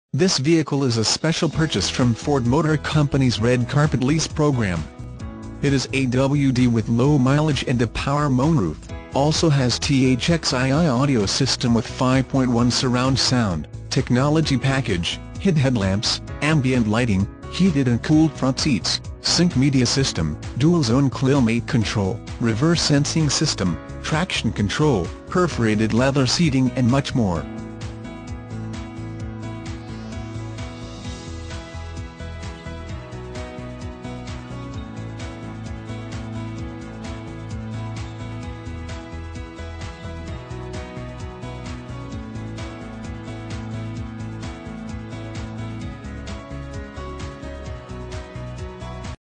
2010 LINCOLN MKZ Dedham, MA 3165P
2010 LINCOLN MKZ Dedham, MA
Stock# 3165P

 This vehicle is a special purchase from ford motor company's red carpet lease program. It is AWD with low mileage and a power monroof, also has thx-II audio system with 5.1 surround sound, technology package, hid headlamps, ambient lighting, heated and cooled front seats, sync media system, dual zone clilmate control, reverse sensing system, traction control, perforated leather seating and much more.